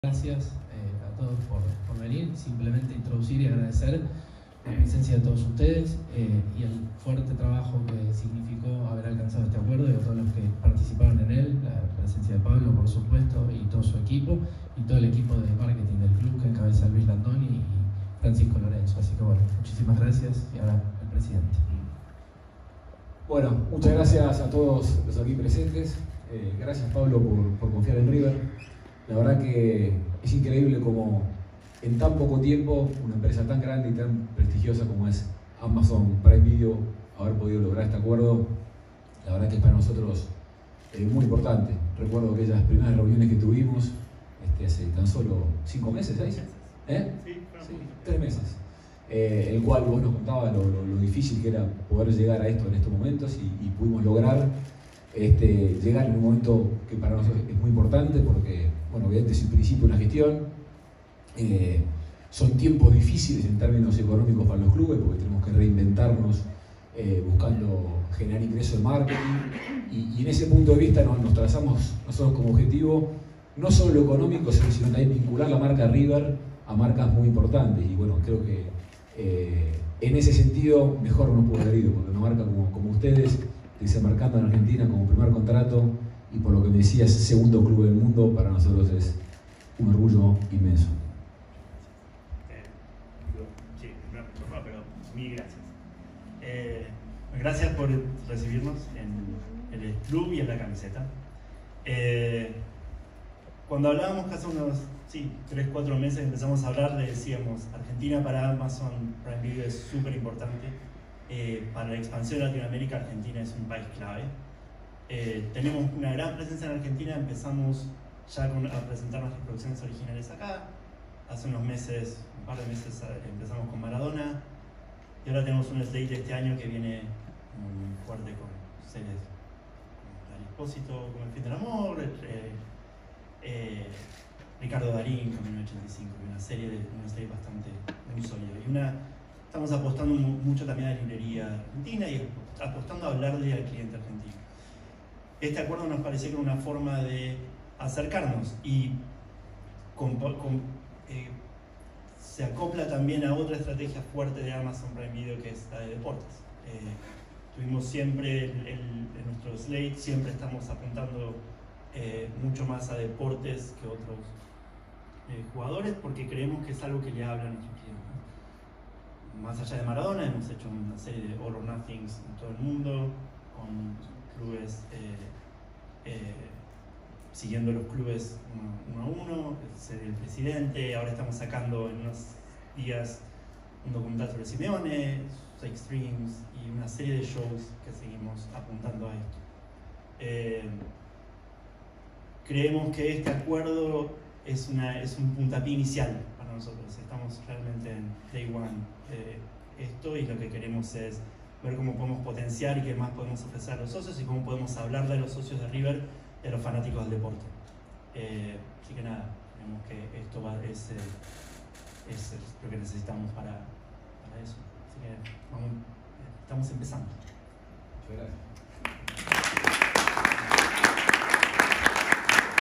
Presentación oficial del acuerdo comercial River-Amazon Prime Video
En un evento organizado en el Estadio Monumental, El Presidente de River Jorge Brito, el Secretario Gral Stefano Di Carlo y el Presidente de Amazon Latam Pablo Lacoviello, presentaron el acuerdo que vincula a ambas marcas a partir de 2022.